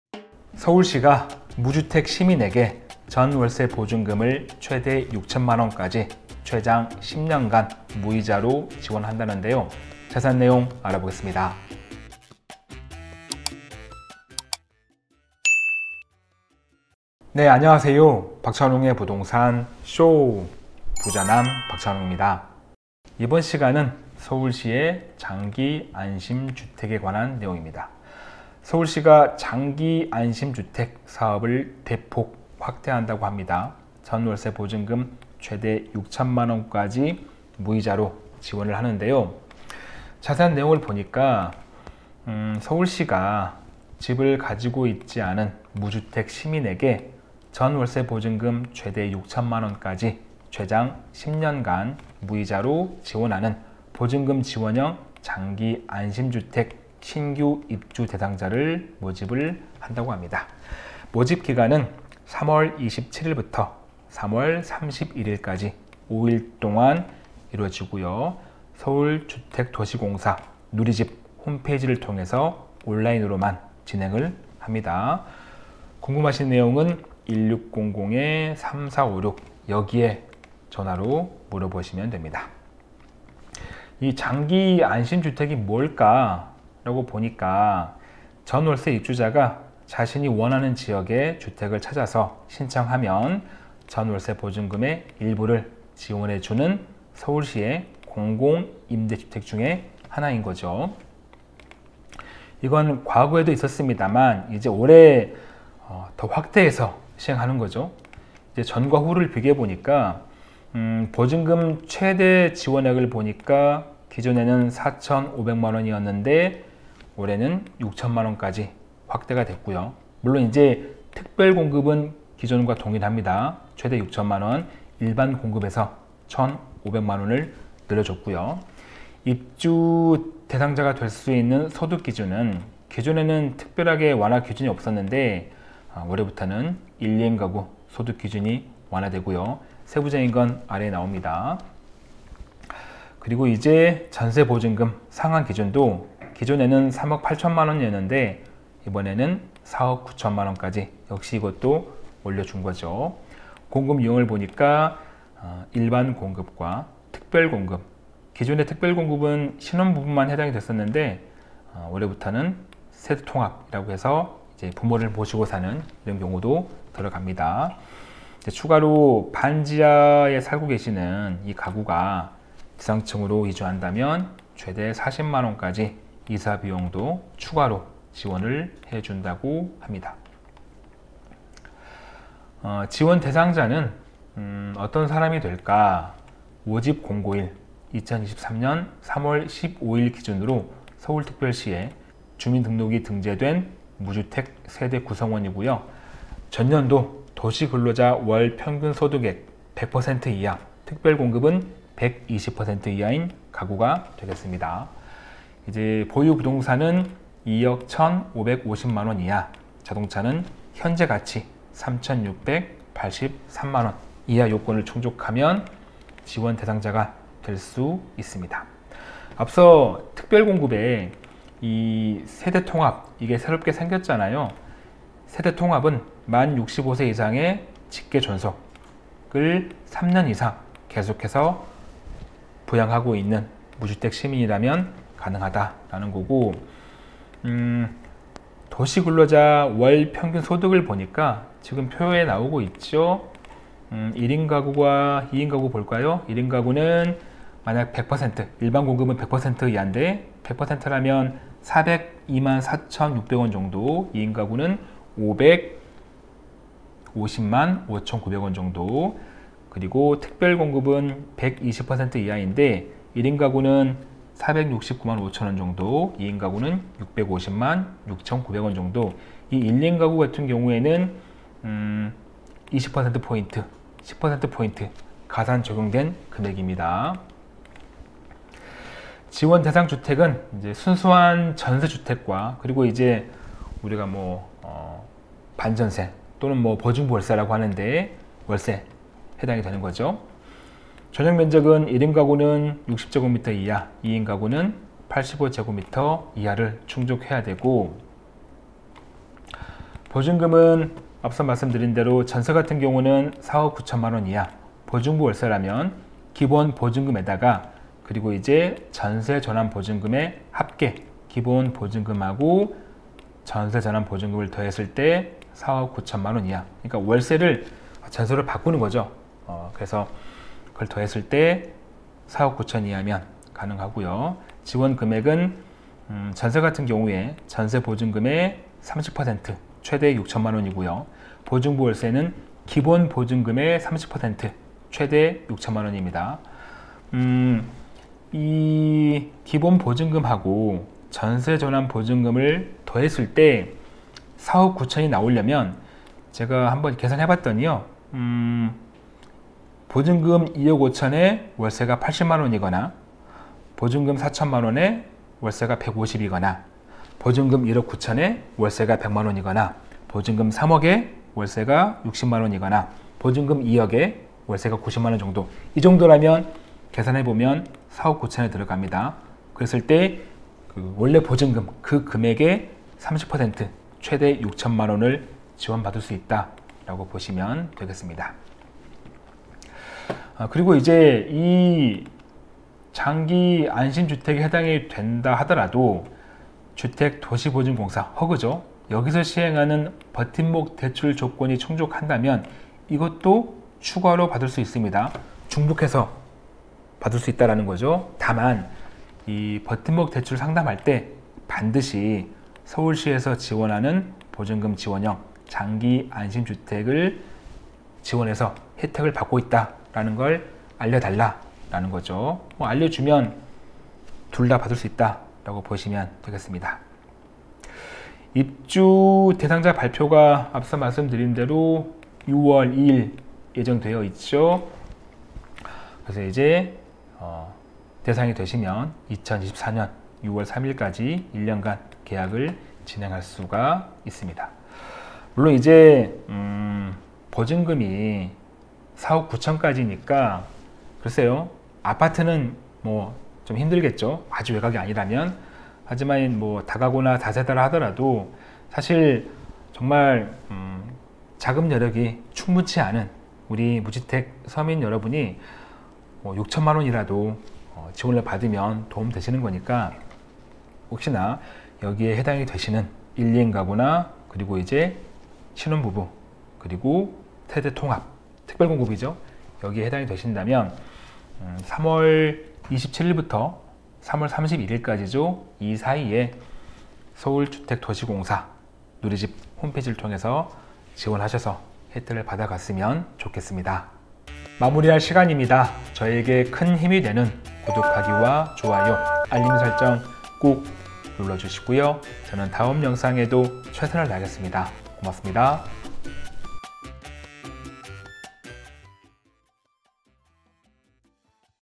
보증금 최대 6,000만원 10년간 무이자 지원! 서울시 장기안심주택
서울시가 무주택 시민에게
전월세 보증금을 최대 6천만원까지,
최장 10년 간 무이자 지원한다는데요,
자세한 내용 알아보겠습니다.